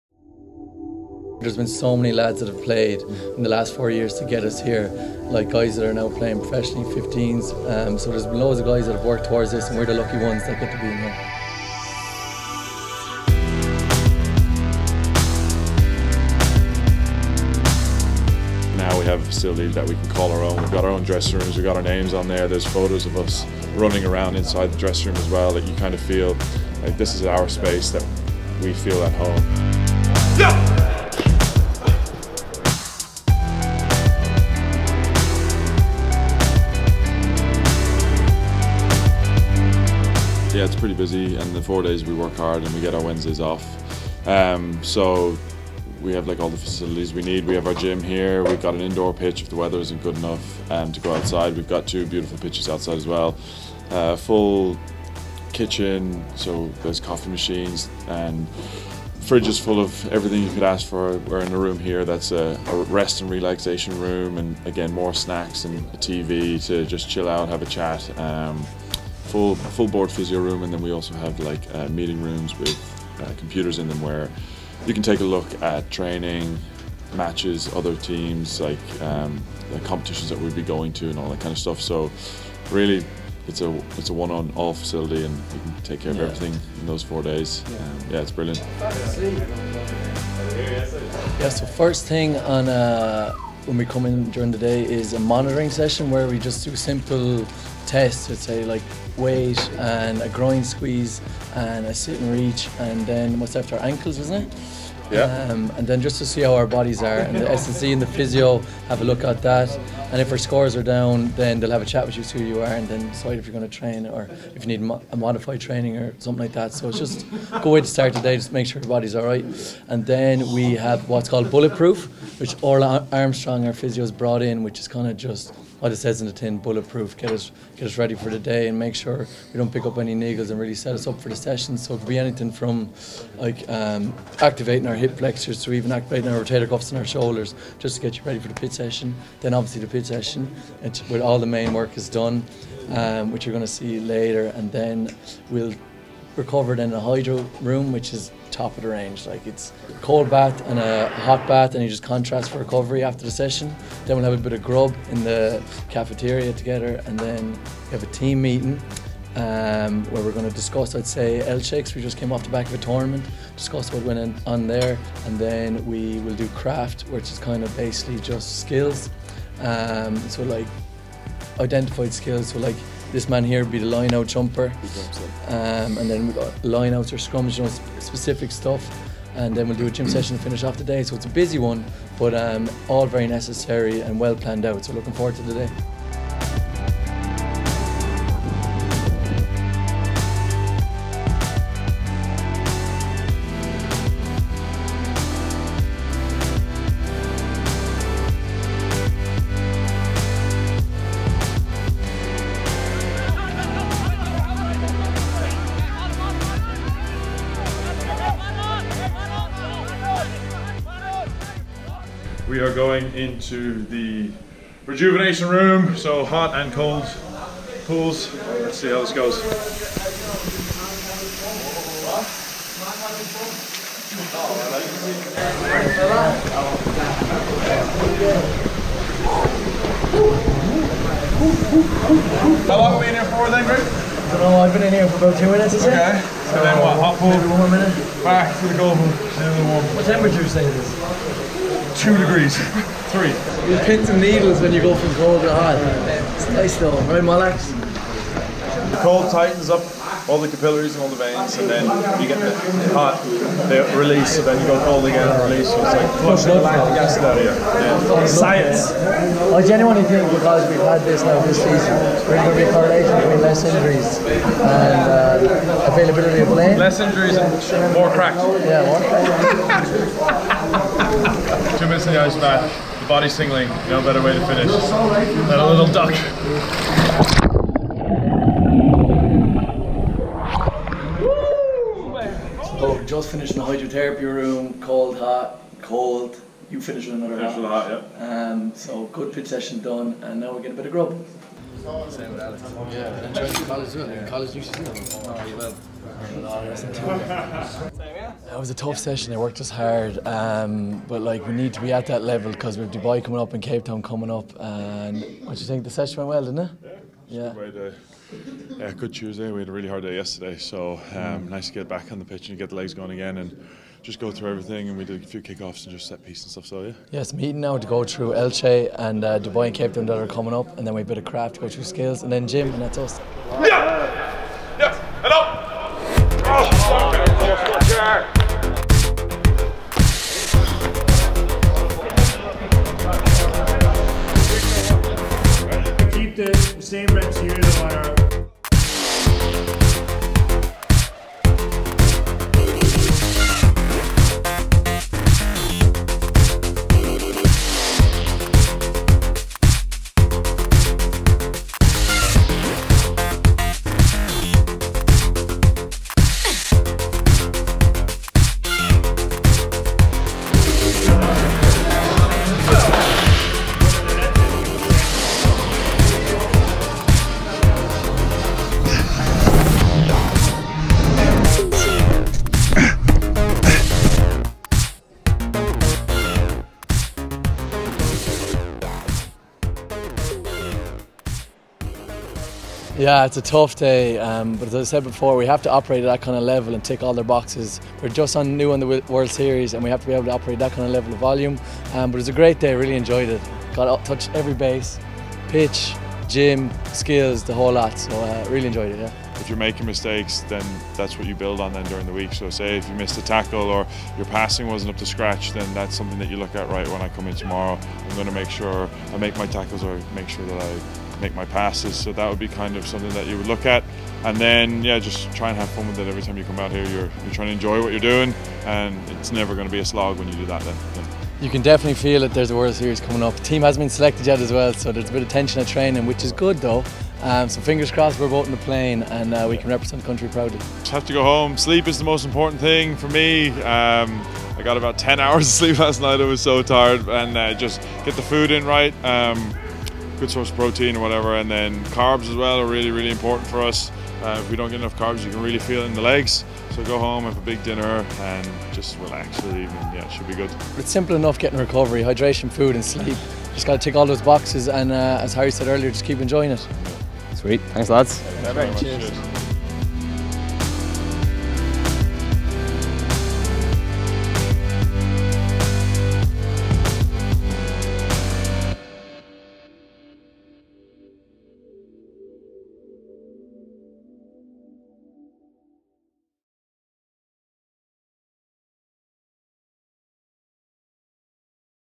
A Day in the Life of an International Rugby 7s player
A day in the life of an Irish rugby 7s player!
This video brings you through a normal training day for an international 7s player; everything from our daily morning monitoring to pitch sessions. Take a look and see how it all operates!

Greg heart ♥️